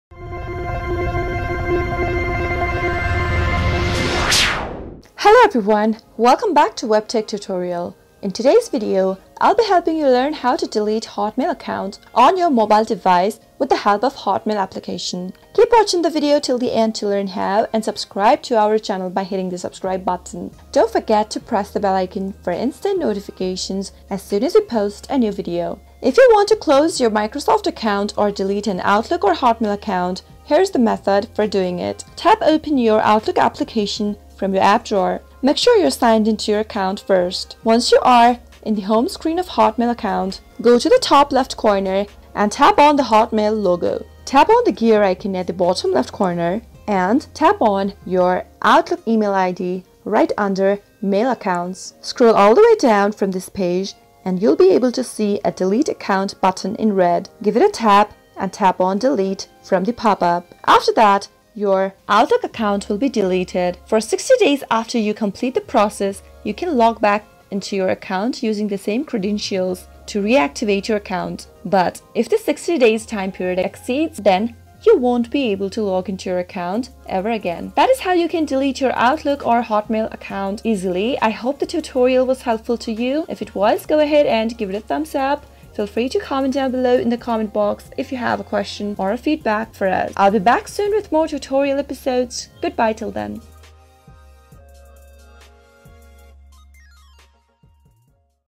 How to Delete Hotmail Account Permanently?
Want to delete your Hotmail account permanently and don't know how? This video might help. Here in this video we are going to show you step by step process on how you can delete your Hotmail account. Just follow some simple steps.
Deleting a Hotmail or Outlook account also deletes all the services associated with it including your saved one drive files. And you won't be able to use it to Sign in to the Microsoft products and services you've been using.

Follow the given steps in order to delete your Hotmail account:
1: Open Outlook app on your device and sign in to your account
2: Tap on the Outlook icon/logo on the top left corner of your screen
3: Tap on the gear icon that is the 'settings' button
4: Tap on your Outlook email account under 'mail account'
5: Tap on 'delete account' and tap on 'delete' from the pop uo and your Hotmail/Outlook account will be deleted
You can log back in to your account using the same login ID till 60 days and if you fail to log in during the 60days time period your account will be deleted permanently.